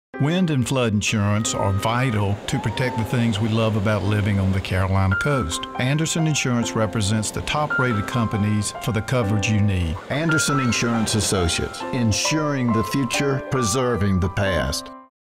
Anderson Insurance Image TV
This ANDERSON INSURANCE IMAGE TV COMMERCIAL promoted wind and flood insurance.